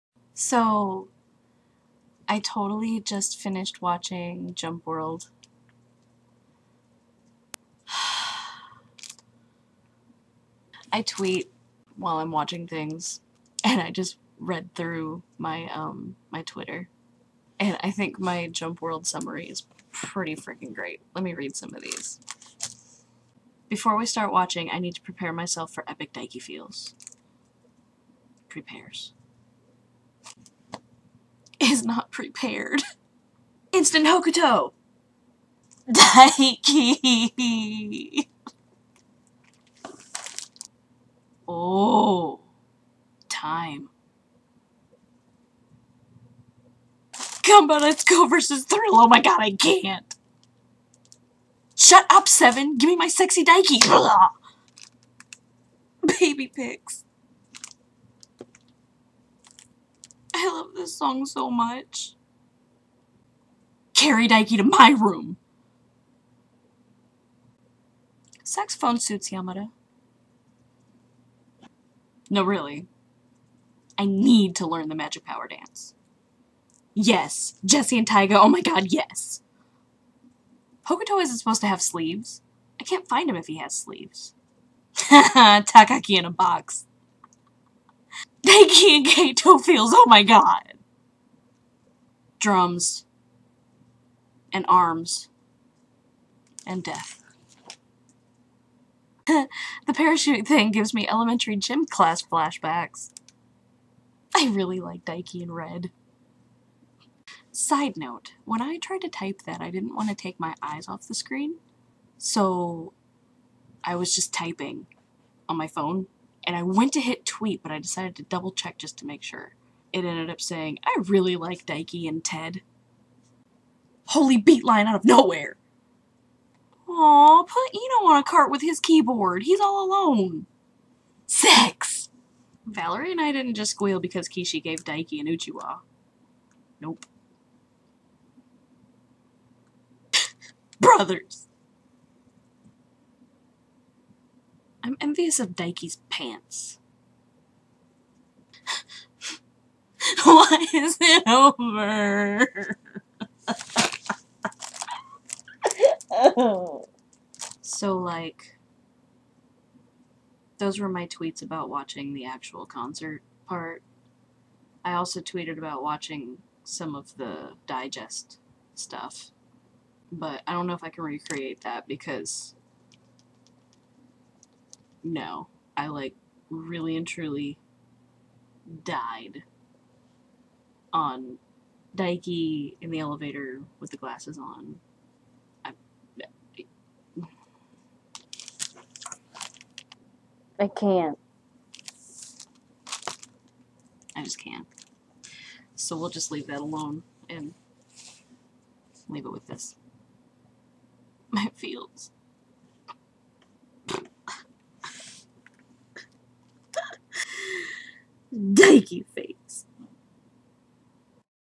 Jump World tweets
I read through the tweets I wrote while watching Jump World and decided they were fantastic, so I made this video XD

.I'm not Daiki biased or anything.